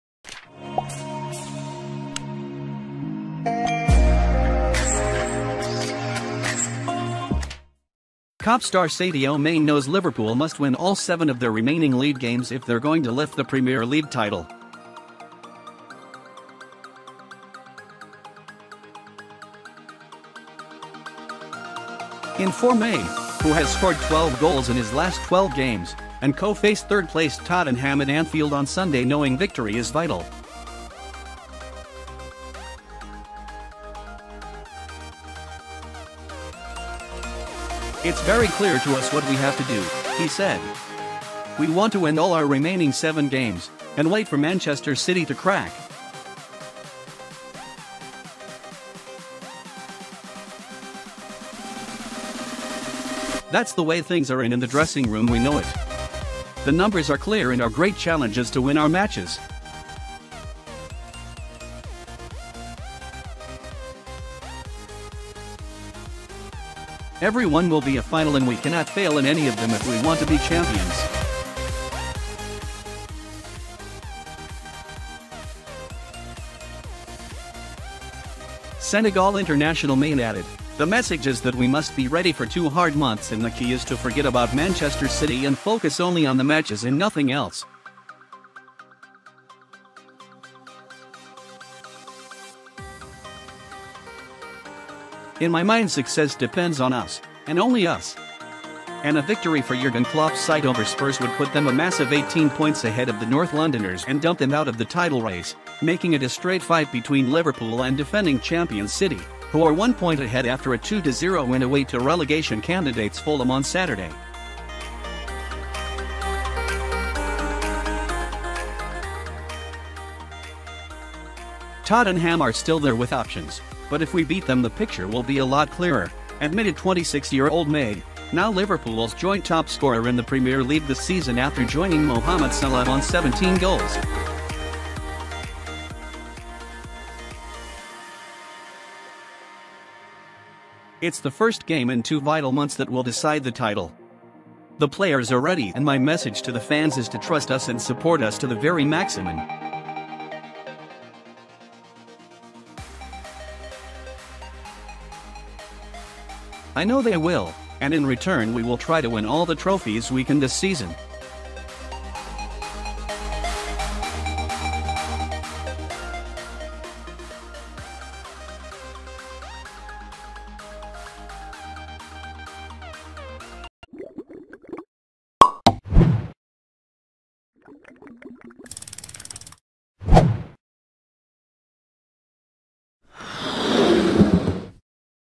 Sadio Mane sets Liverpool team-mates a tough target for title race run-in Man City News
Kop star Sadio Mane knows Liverpool must win all seven of their remaining league games if they’re going to lift the Premier League title.    In-form Mane, who has scored 12 goals in his last 12 games, and co face third-placed Tottenham at Anfield on Sunday knowing victory is vital.   “It’s very clear to us what we have to do,” he said. “We want to win all our remaining seven games, and wait for Manchester City to crack. That’s the way things are and in the dressing room we know it.  “The numbers are clear and our great challenge is to win our matches. Every one will be a final and we cannot fail in any of them if we want to be champions.     Senegal international Mane added: “The message is that we must be ready for two hard months and the key is to forget about Manchester City and focus only on the matches and nothing else. In my mind success depends on us – and only us.”    And a victory for Jurgen Klopp’s side over Spurs would put them a massive 18 points ahead of the north Londoners and dump them out of the title race, making it a straight fight between Liverpool and defending champions City, who are one point ahead after a 2-0 win away to relegation candidates Fulham on Saturday.   “Tottenham are still there with options, but if we beat them the picture will be a lot clearer,” admitted 26-year-old Mane, now Liverpool's joint-top scorer in the Premier League this season after joining Mohamed Salah on 17 goals.  “It’s the first game in two vital months that will decide the title. The players are ready and my message to the fans is to trust us and support us to the very maximum.  “I know they will – and in return we will try to win all the trophies we can this season.”